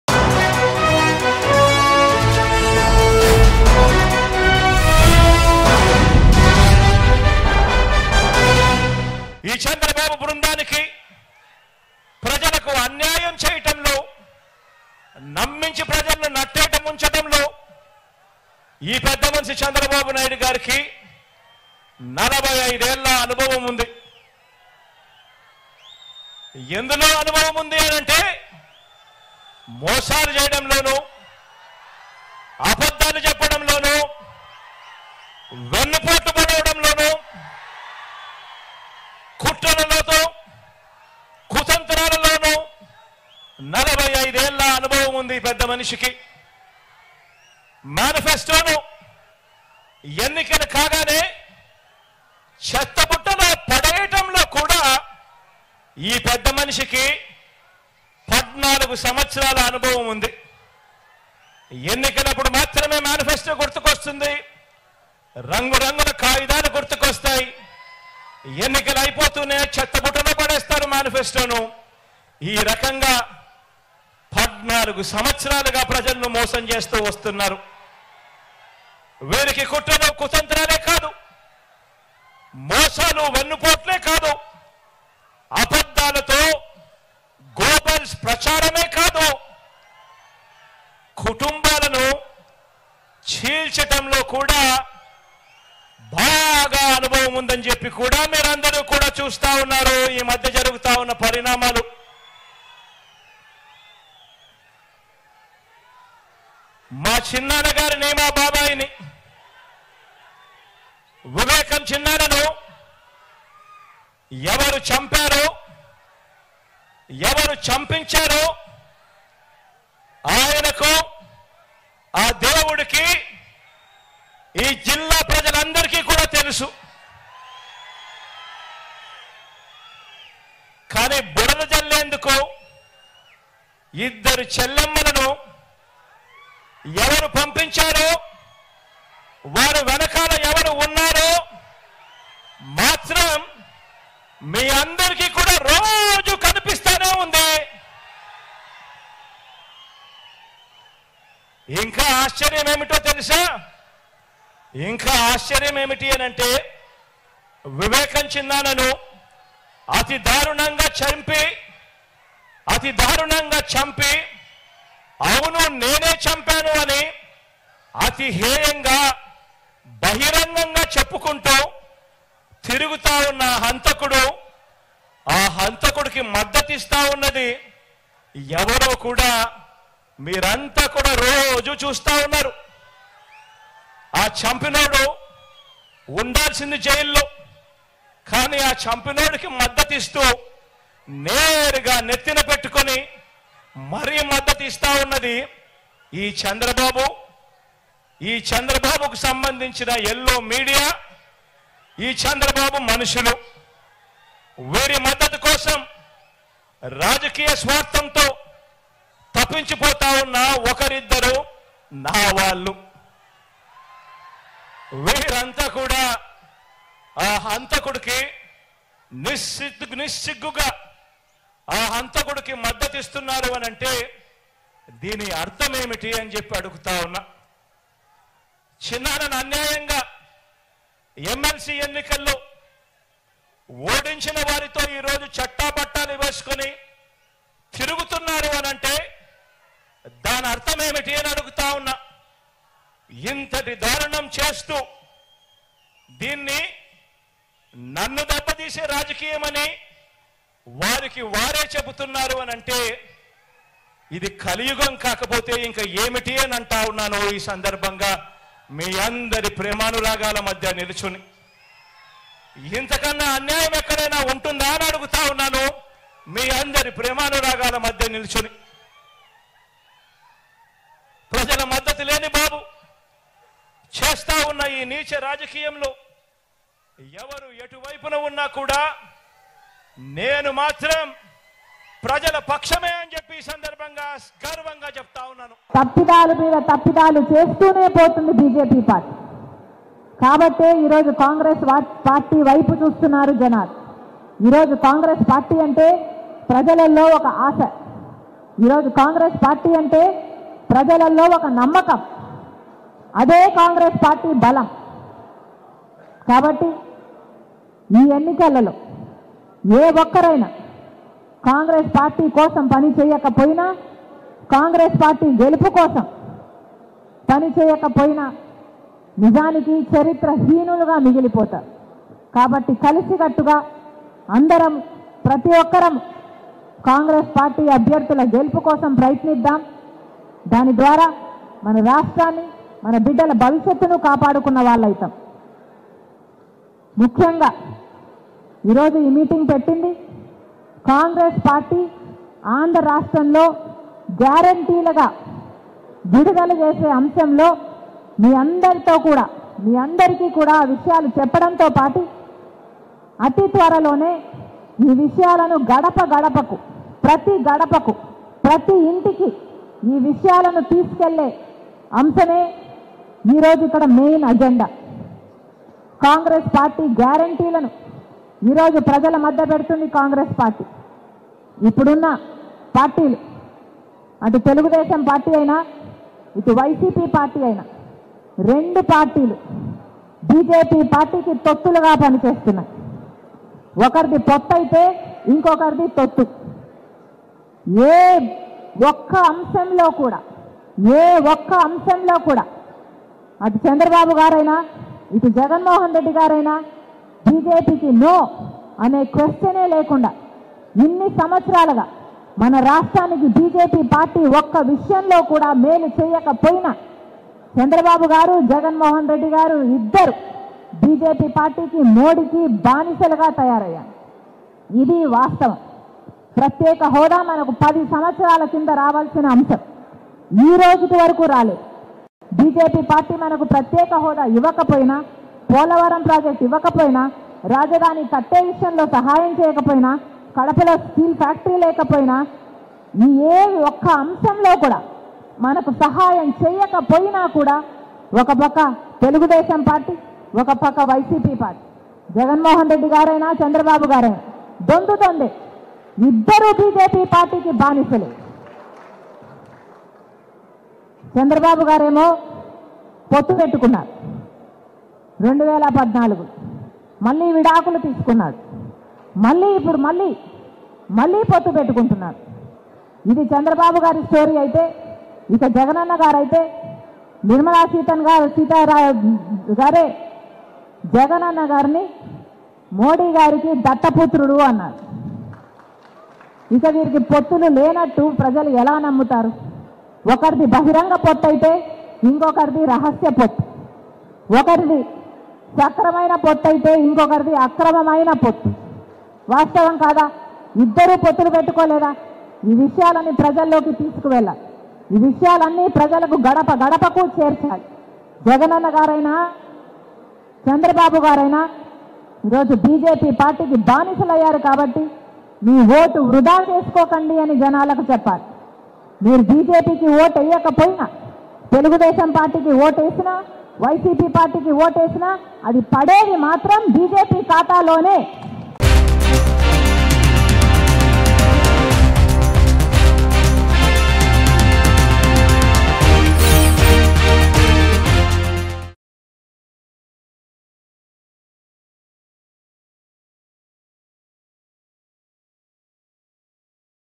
Heated Argument Between CM YS Jagan And YS Sharmila | YCP Vs Congress | AP News | Mango News
Heated Argument Between CM YS Jagan And YS Sharmila. Andhra Pradesh Elections On May 13th.